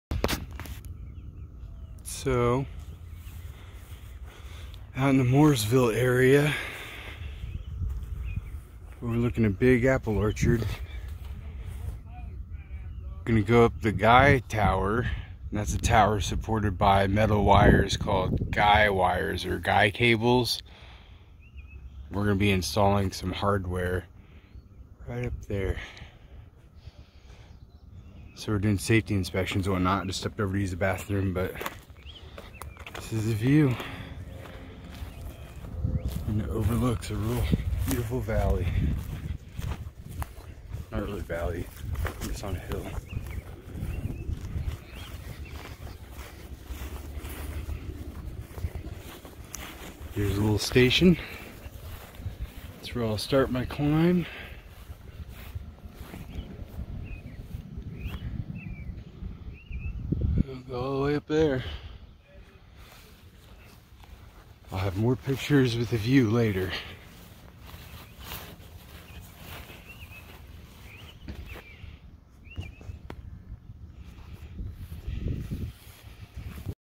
Tour of the tower
we added an antenna and secured the accompanying cable 150 feet up the face of a Guy Tower.